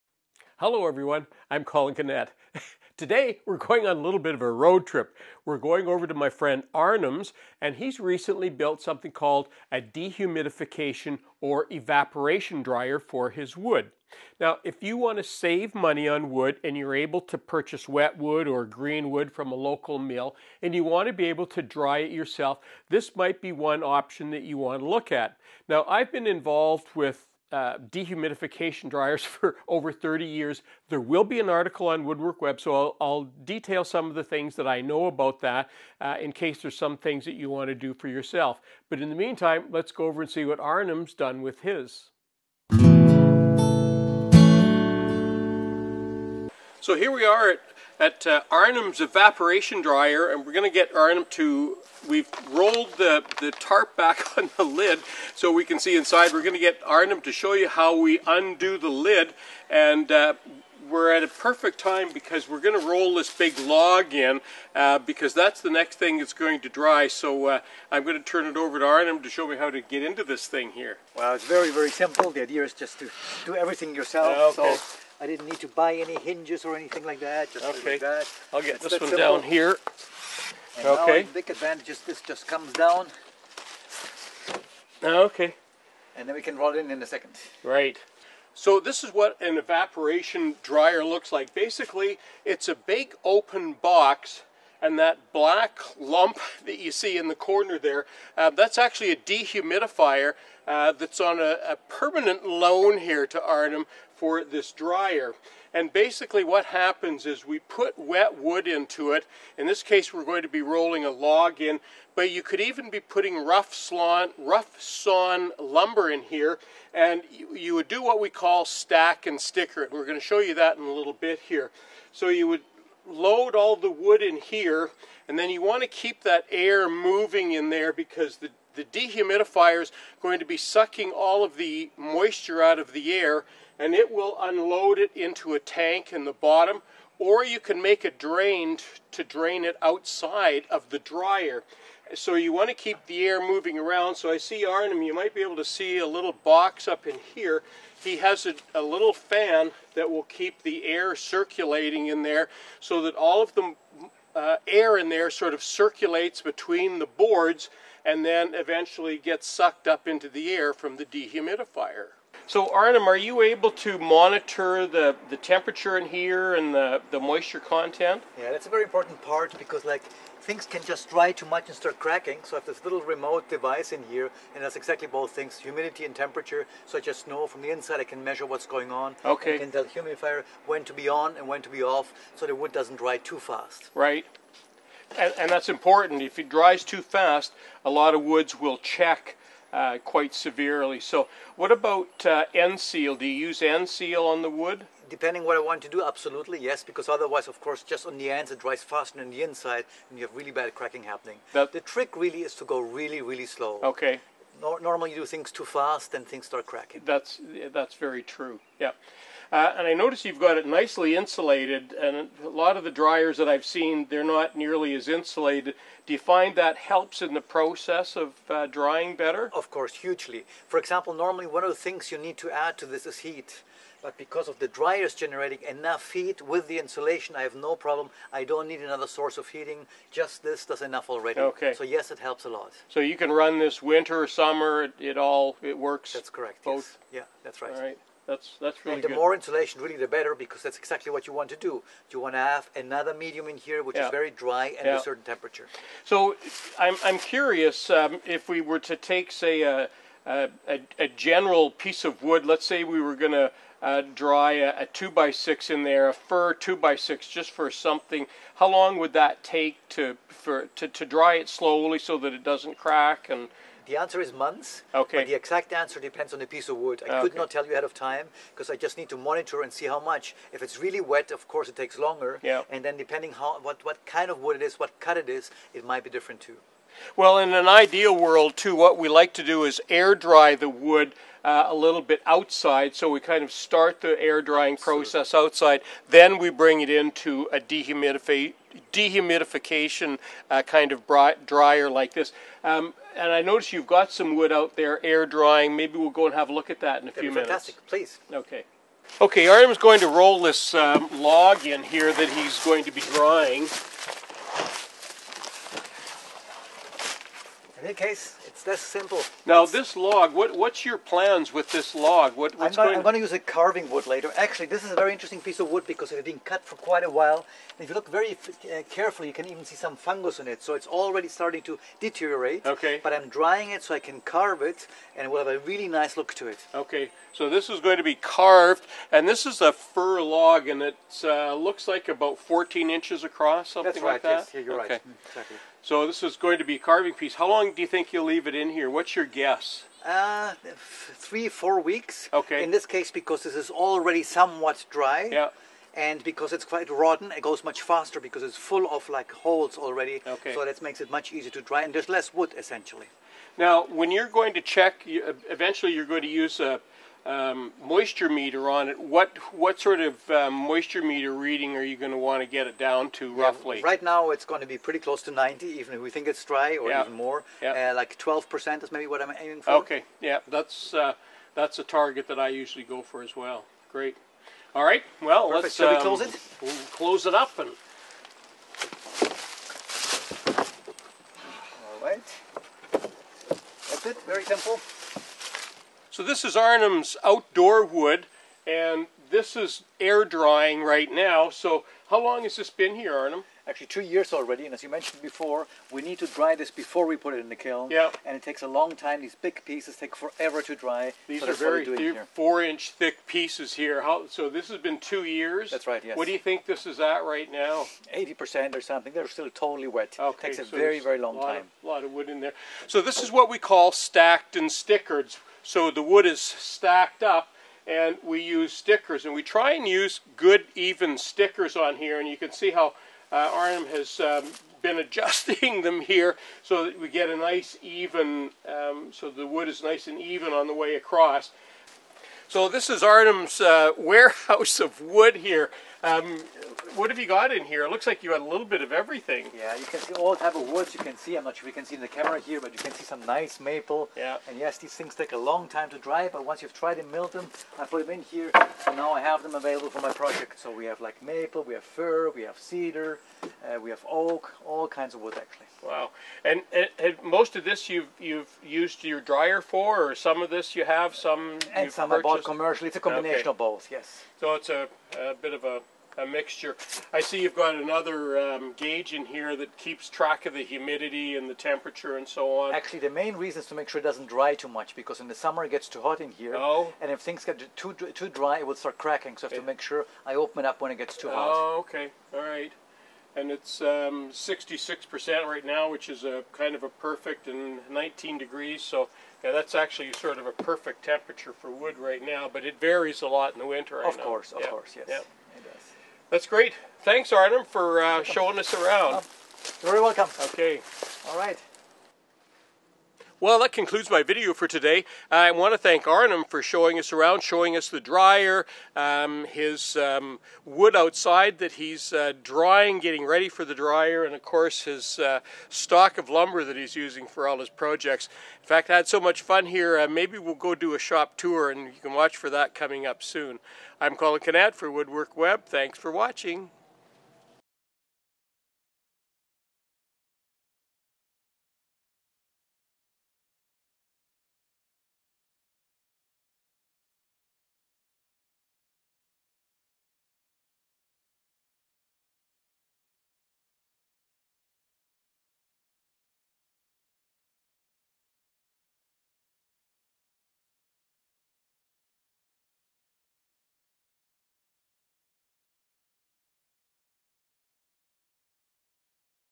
Drying Wood with Dehumidifier Wood Kiln - How to Dry Wood
In this video, I show you how to dry wood using a dehumidifier wood kiln.  Drying wood in your own kiln or wood dryer is a great way of getting speeding up the drying process and is an easy project to make with some construction grade plywood or even OSB sheets, add a dehumidifier when it's finished and now you have your own wood dryer/kiln